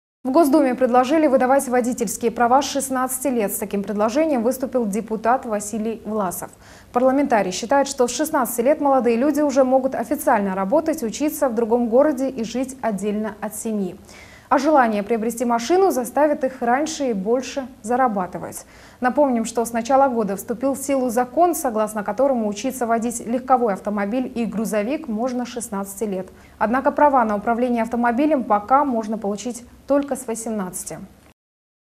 Дорогу молодым: стоит ли разрешать водить с 16 лет
В Госдуме предложили выдавать водительские права с 16 лет. С таким предложением выступил Депутат Василий Власов.

Парламентарий считает, что с 16 лет молодые люди уже могут официально работать, учиться в другом городе и жить отдельно от семьи. А желание приобрести машину заставит их раньше и больше зарабатывать.

Напомним, что с начала года вступил в силу закон, согласно которому учиться водить легковой автомобиль и грузовик можно с 16 лет. Однако права на управление автомобилем пока можно получить только в 18 лет.